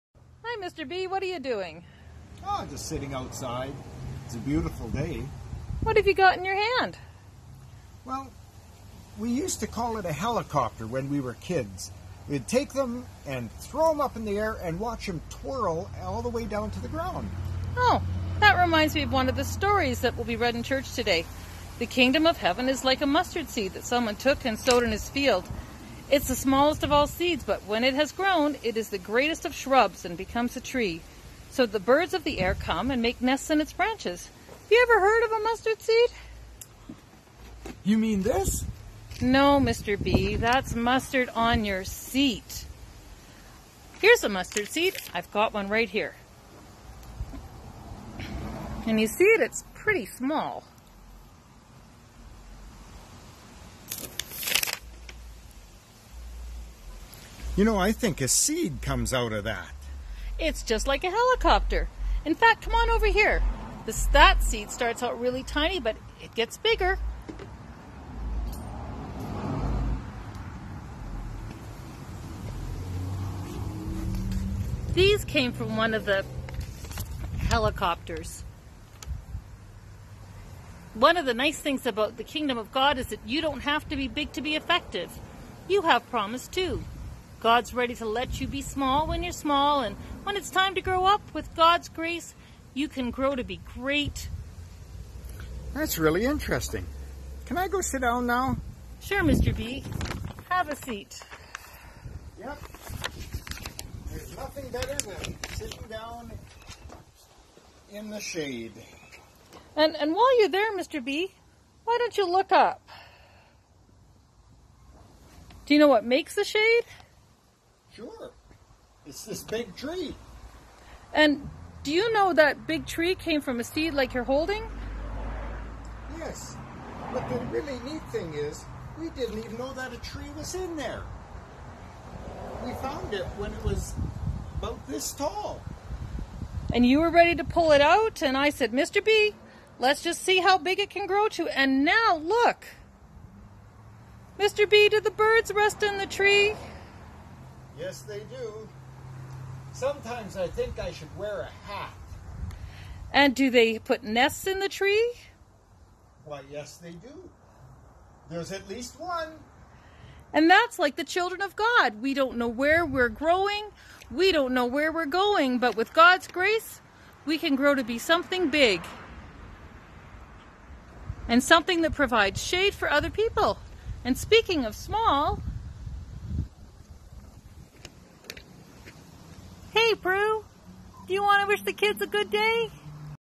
July 26th Children's Focus
Learn about the parable of the mustard seed with the Besplug's!